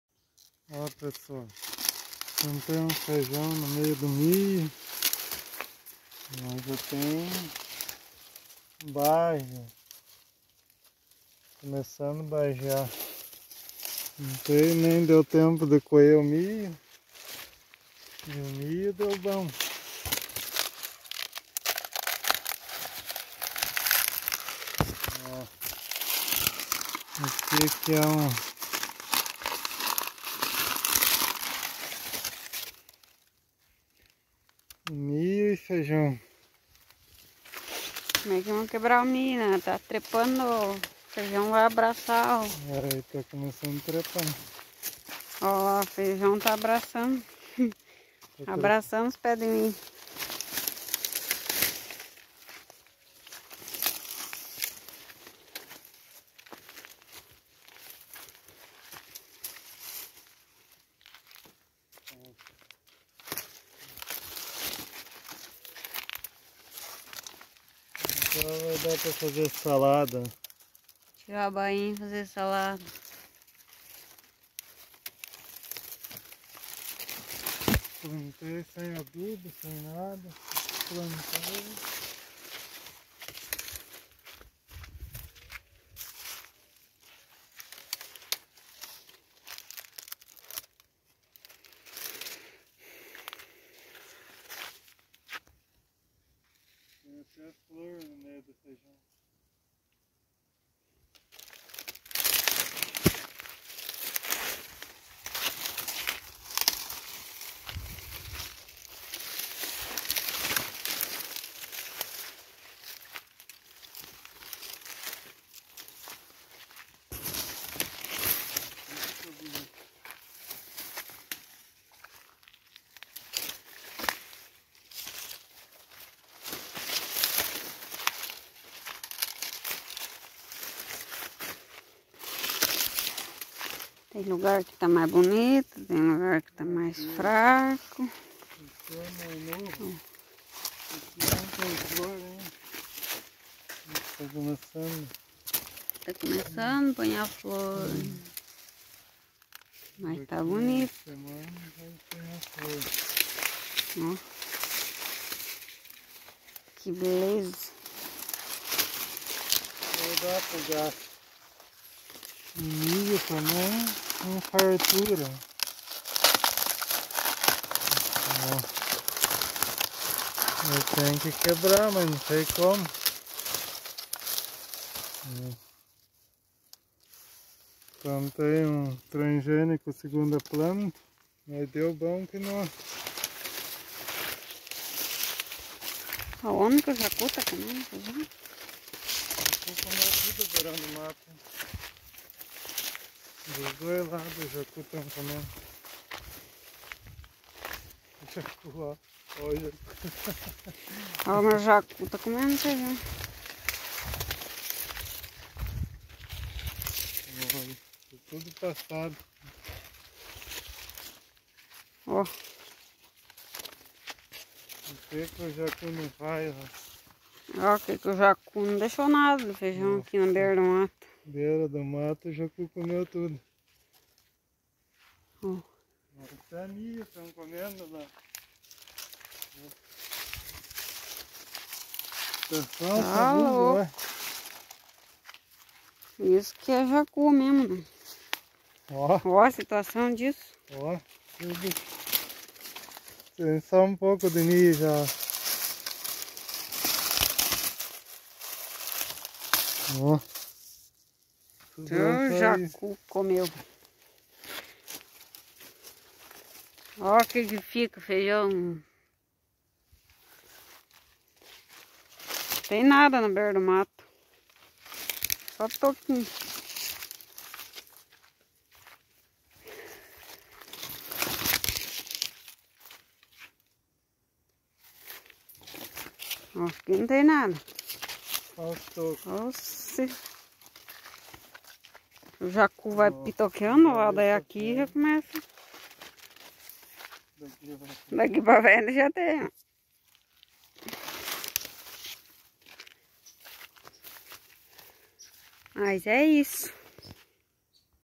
Nosso feijão no milho 🌽, mas tem jacu estragando...
fomos ver nosso feijão, porém os jacu estão comendo.